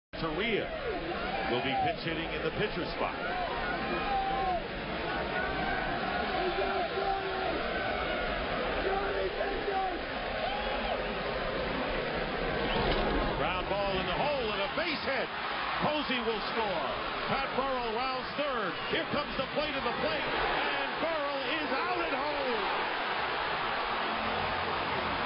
OLD SCHOOL BASE BLOCK AT HOME
Old school tag play @ home.Catcher in line to receive the throw. Worth noting that catcher rolls with contact & shows umpire that he is in control of the ball.